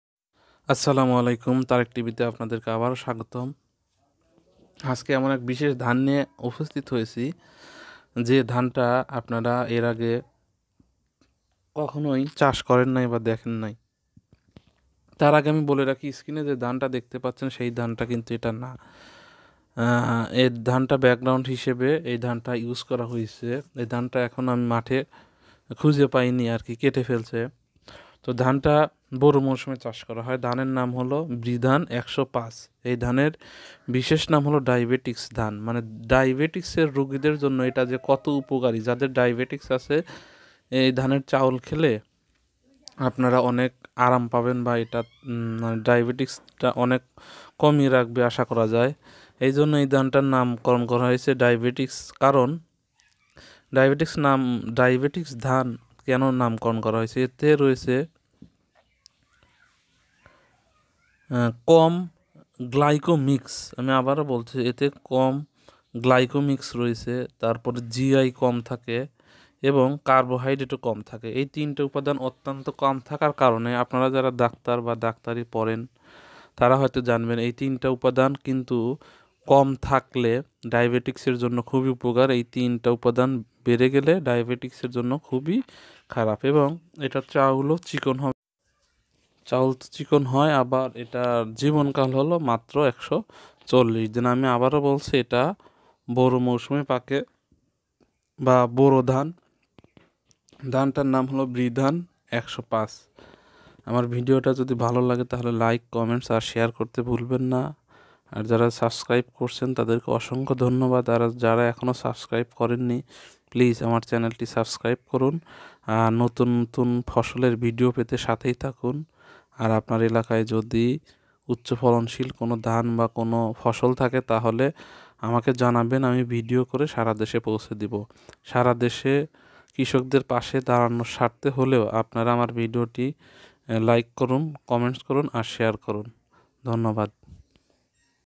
ডাইবেটিস ধান। কেন চাষ করবেন।জানুন ধানটি সম্পর্কে ।
ডাইবেটিস ধান:

বিজ্ঞানীরা বলছেন এবারের ধানটি উচ্চ ফলনশীল জাতের এবং হেক্টরপ্রতি এর সম্ভাব্য উৎপাদন সাত টনের বেশি হবে বলে আশা করা হচ্ছে। ব্রি বলেছে, বোরো মৌসুমে চাষের উপযোগী ব্রি ধান ১০৫ থেকে পাওয়া চালে কার্বোহাইড্রেটের পরিমাণ তুলনামূলক কম এবং সে কারণেই একে 'ডায়াবেটিক ধান' বলা হচ্ছে।

ফলো মি:
ফেসবুক আইডি: